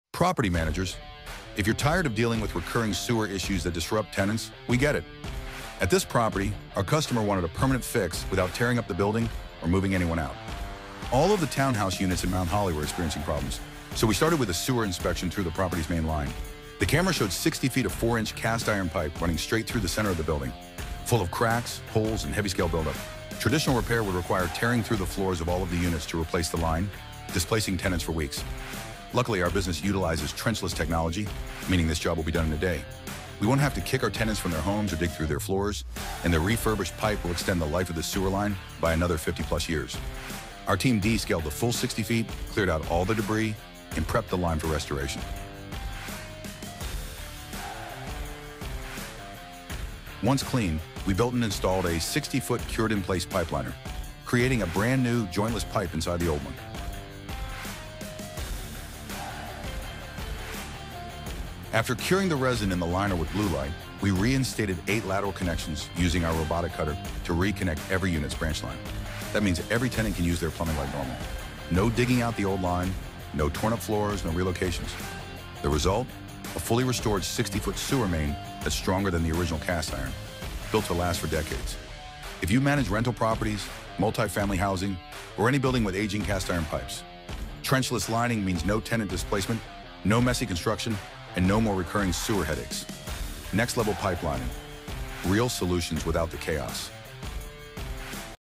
Pipe Lining For Property Managers, Landlords, and Complex Owners in North Carolina | Sewer Repair
This townhouse community had a failing main sewer line running under several units. Instead of digging through floors, sidewalks, or landscaping, we restored the entire line with trenchless pipe lining: fast, clean, and built to last.
Perfect for:
 • Property Managers
 • Landlords
 • HOAs
 • Multi-unit buildings & townhome communities
Avoid demolition, avoid downtime, and protect your property with long-term trenchless solutions.